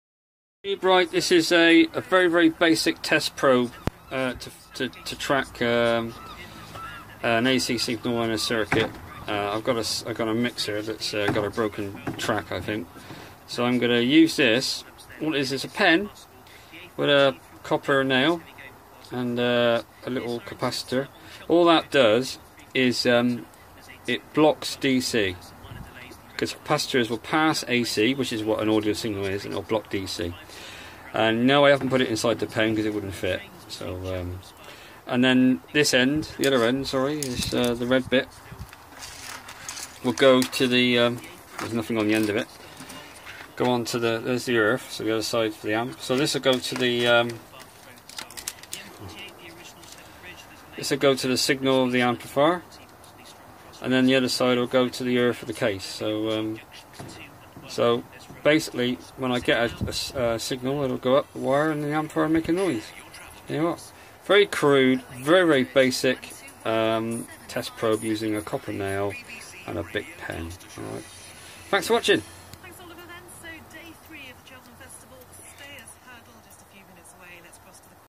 Pcb signal tracer probe
Very crude signal tracer probe.used a pen case and a capacitor to block dc voltages incase it touches something other than a ac signal.stops amplifier getting damaged